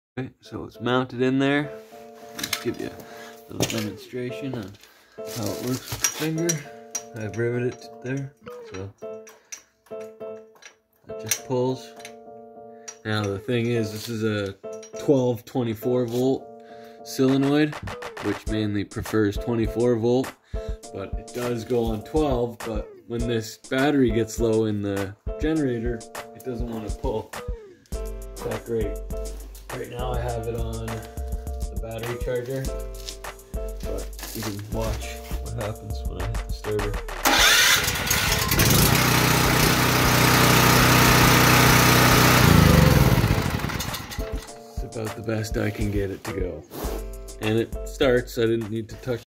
DIY Auto Choke for Portable Generators
A preview to an upcoming video. I'm trying to get this Benchmark BG6250E generator to start from a Magnum inverter with a ME- AGS-N. We will see. I don't think this is recommend by either company but that's why I'm here. It's a good learning experience as well. The full video on the build coming soon.